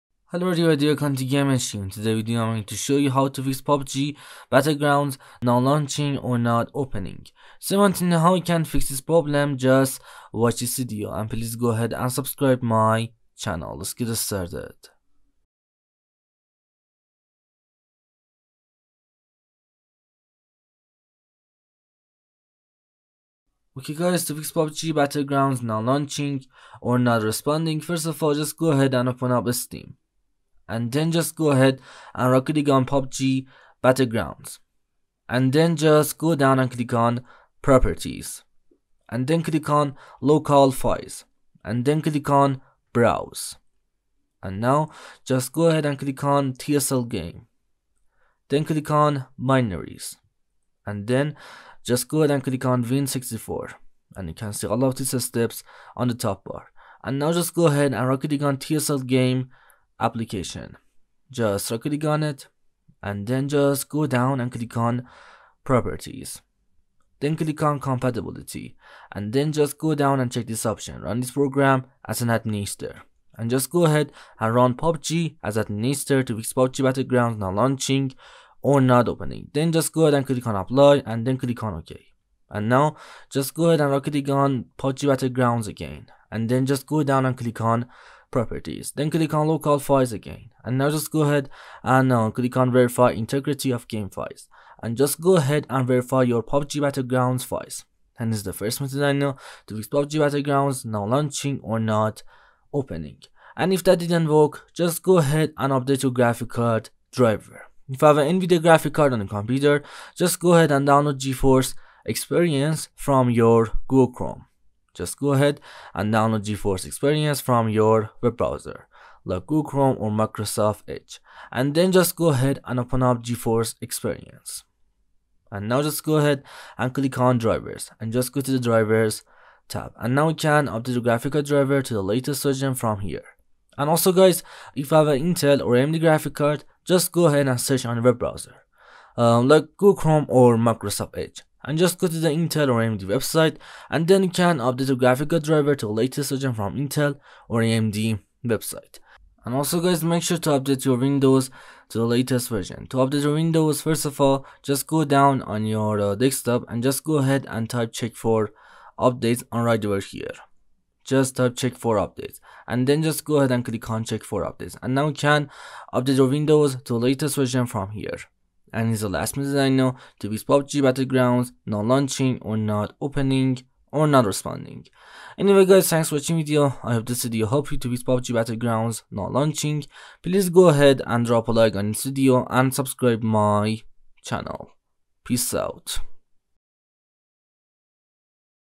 How To Fix PUBG Battleground Not Launching or Not Opening [steam]
Hello guys in this video I'm showing you How To Fix PUBG Battlegrounds Not Launching or Not Opening [steam]. so if you want to know how to fix pubg not working just watch this video.